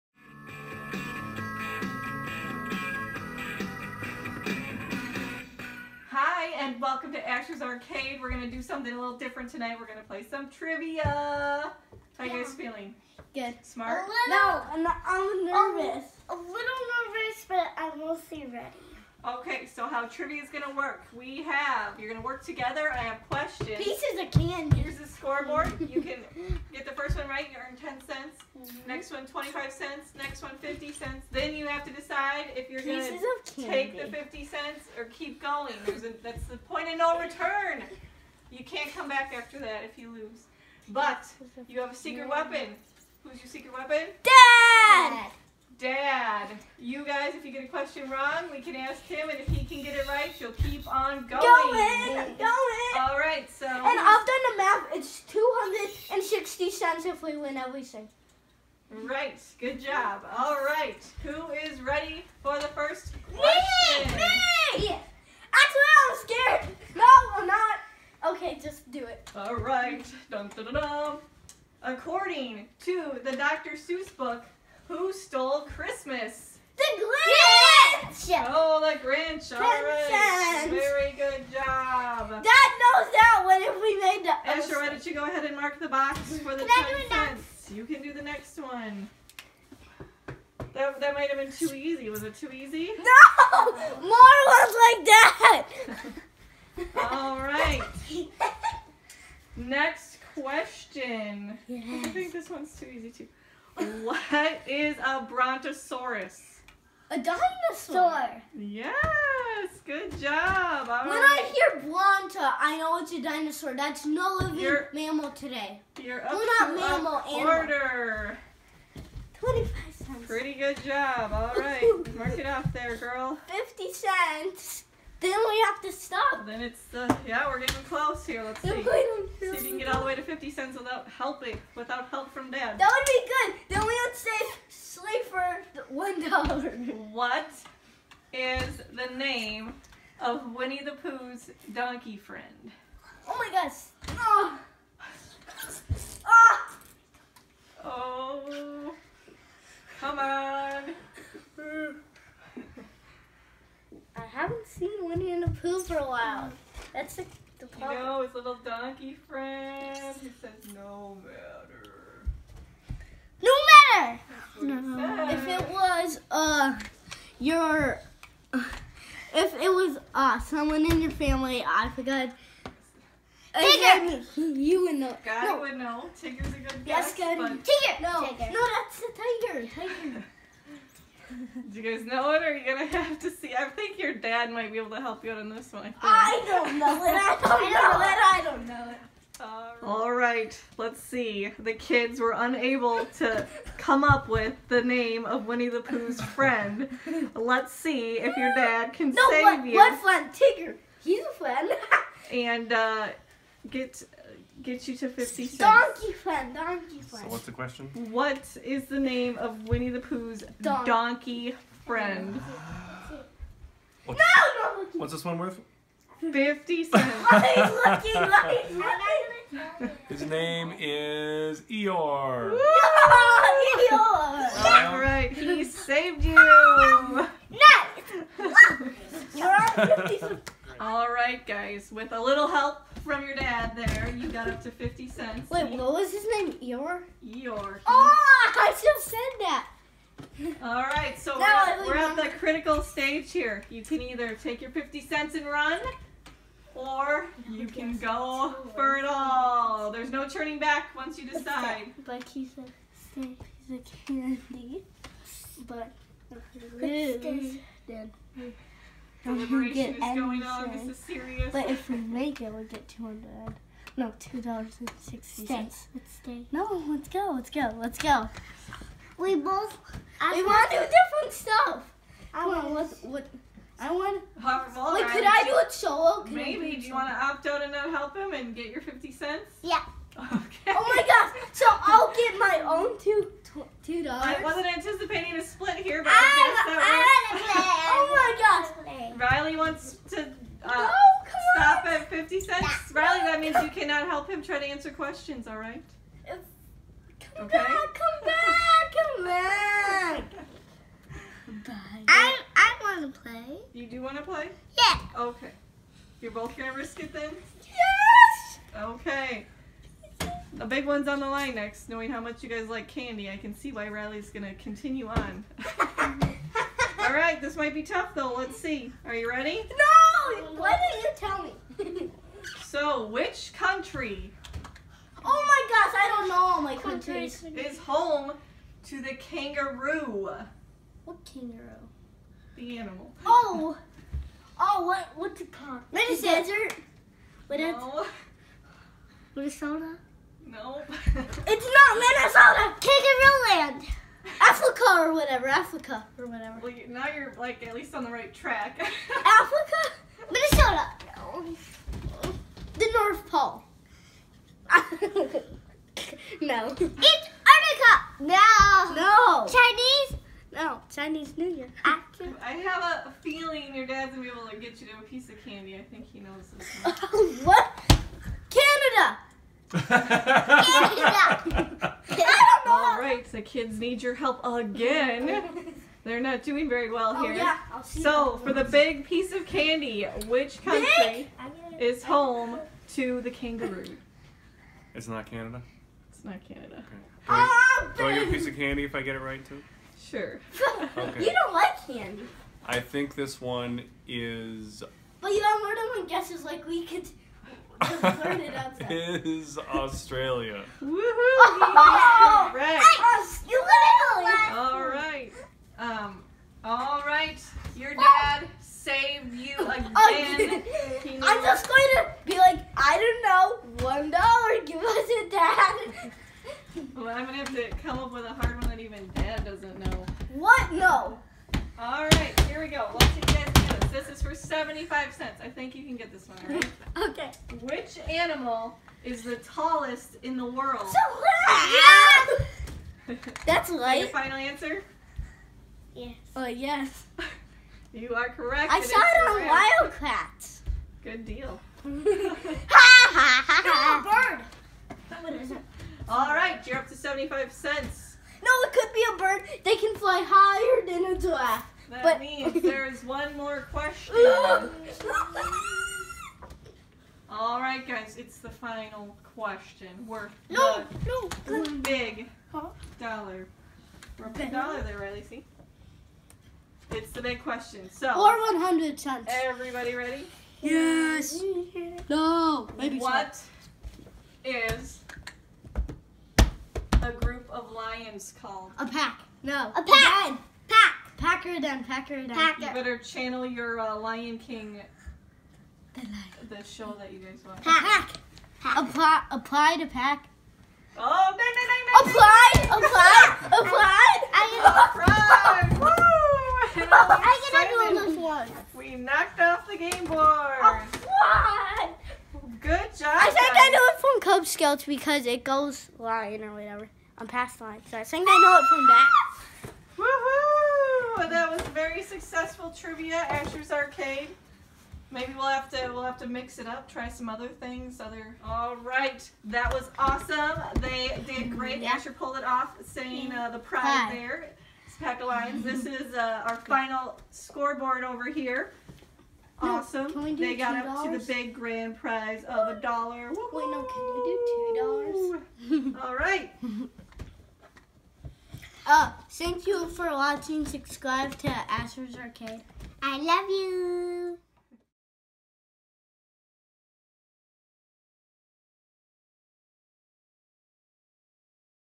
Family trivia night for big prizes!
Asher and Rylee take on the big trivia challenge, can they win it all or will they go to bed empty handed. The game works as they move their way up the prize board, once the earn a piece of candy they can either choose to keep it or go on for the big prize of a $1.00. If they get one wrong they lose it all, except they have a safety net!! If dad can answer the question correctly after they get it wrong, they stay in the game and can go on.